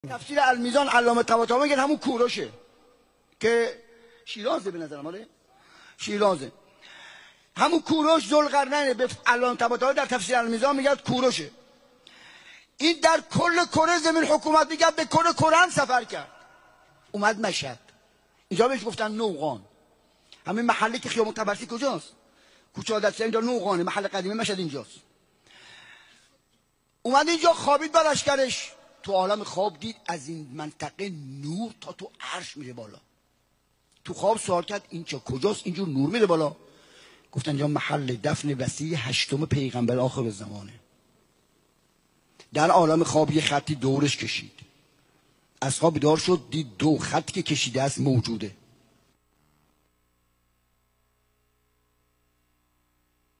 IRAN, روايتي از « کورش » ذوالقرنين در قرآن ـ گور رضا ـ خراسان ؛
IRAN, Dhul-Qarnayn, Quran, Cyrus The Great, Grave, Khorasan, Imam REZA,